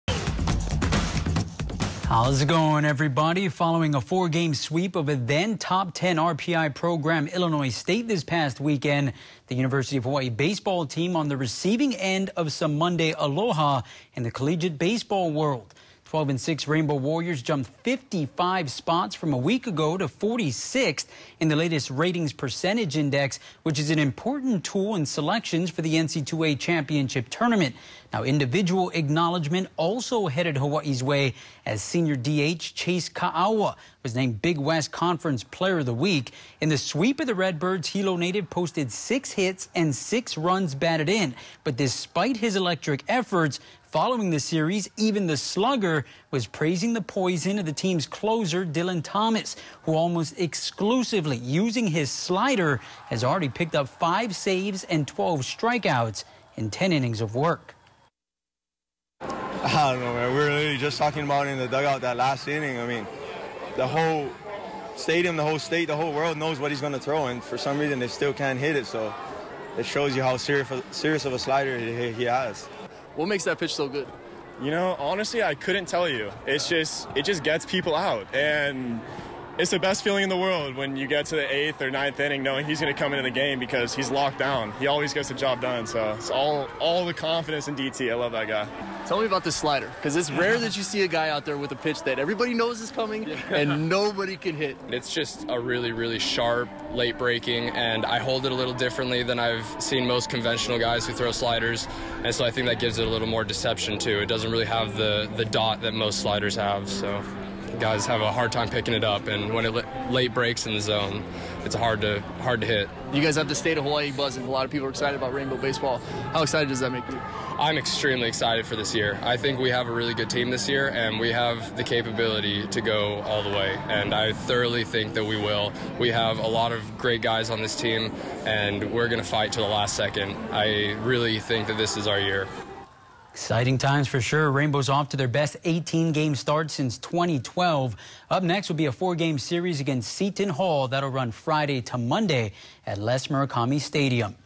'Bows baseball is can't miss for fans while Thomas' slider is can't hit for opponents
The sophomore right hander almost exclusively using his slider as has already picked up five saves and 12 strikeouts in 10 innings of work.